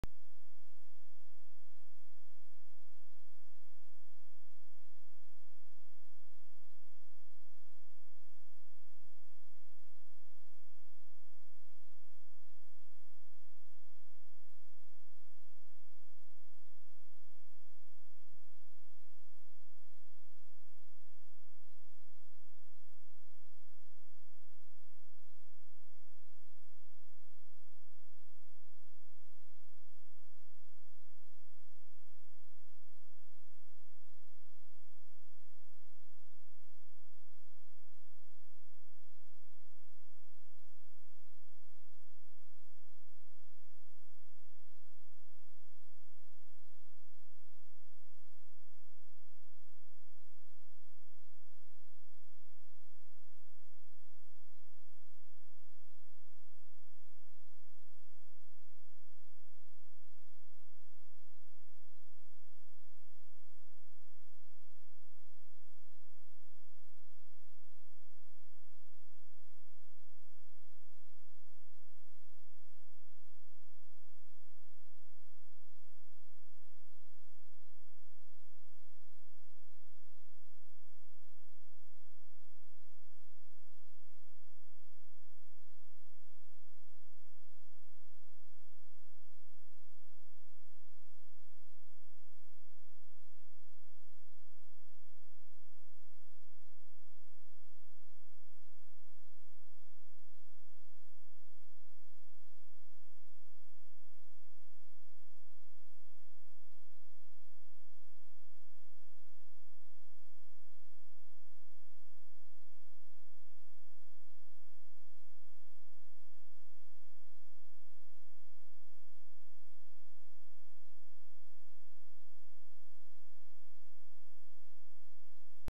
Emisión de vapor de agua volcán submarino El Hierro 24 enero 2012
Impresionantes imágenes de la emisión de vapores por la erupción del volcán submarino frente a La Restinga, El Hierro - Día 24 de enero de 2012